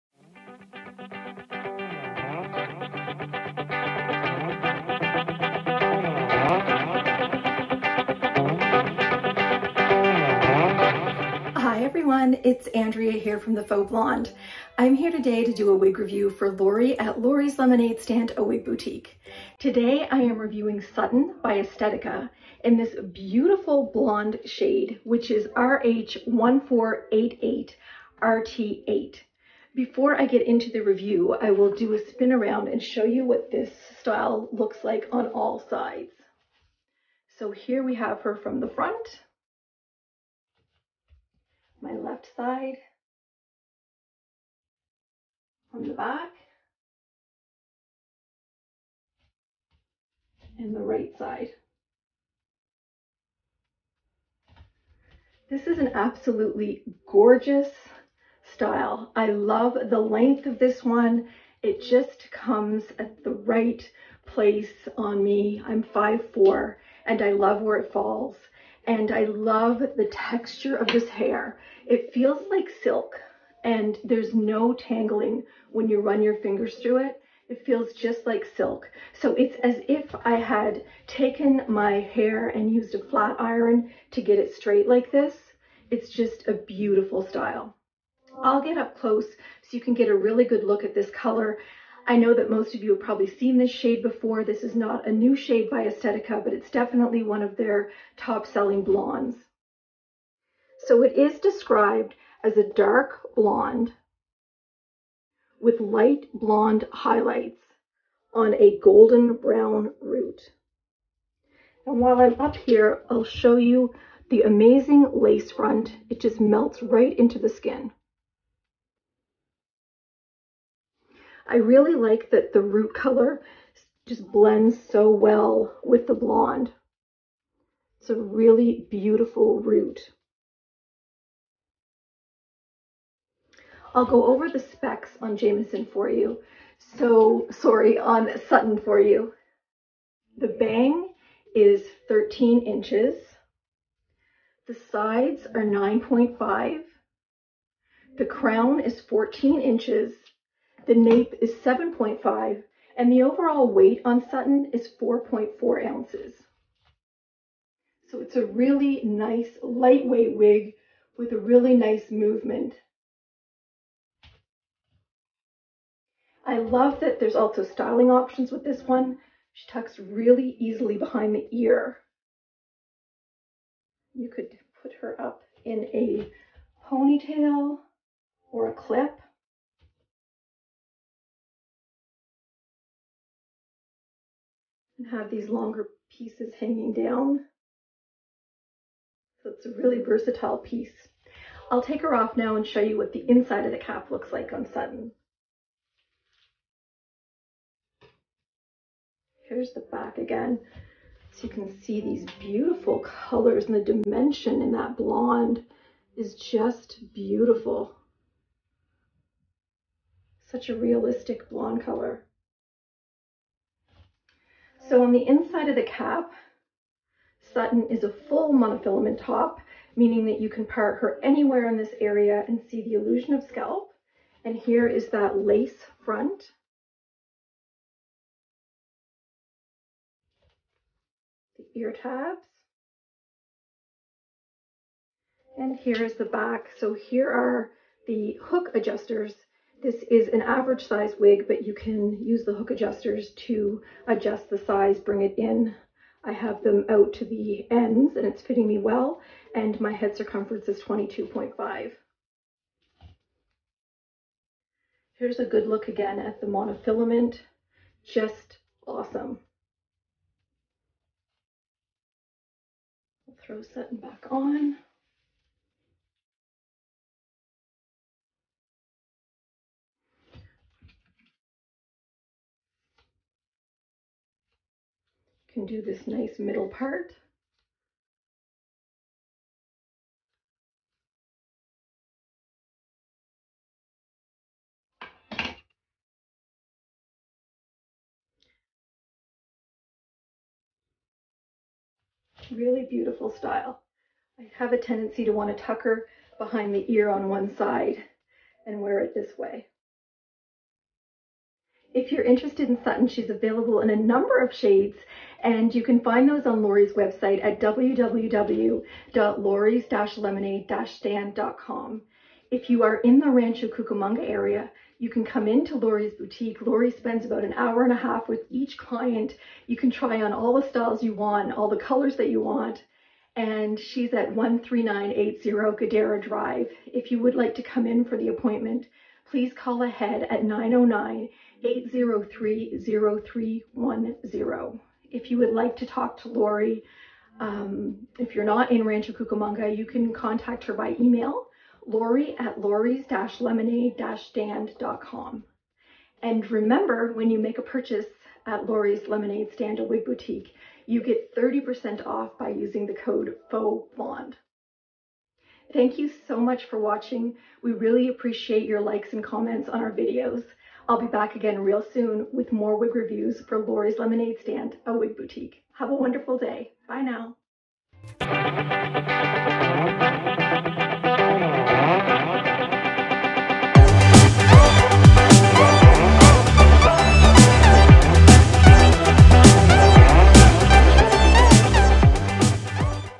WIG REVIEW: Sutton by Estetica in color RH1488RT8
Available at Lori's Lemonade Stand where we want to help you turn your lemons into lemonade!

This straight, blunt cut lob is simple, yet modern. Sutton’s sleek, straight texture and perfectly trim line radiates style. Whether you part with a simple center part or extreme side part, Sutton will always look incredibly polished.

MONOFILAMENT
LACE FRONT | Medium Length Blunt Cut Lob
BANG - 13"
SIDE - 9.5"
CROWN - 14"
NAPE - 7.5"
WEIGHT - 4.44oz

13980 Guidera Drive
Rancho Cucamonga, CA